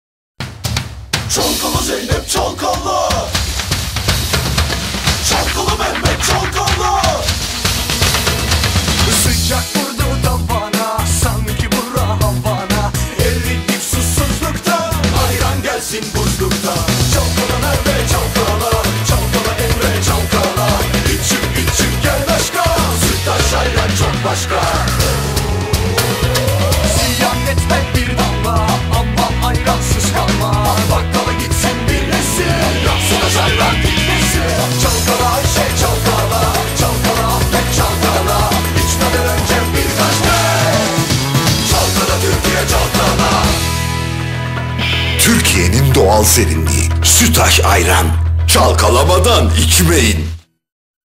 Sutas Ayran Drink "Dance" by Levent Semerci
Sutas Ayran Drink "Dance" commercial
Directed by: Levent Semerci
Production Company: CRVD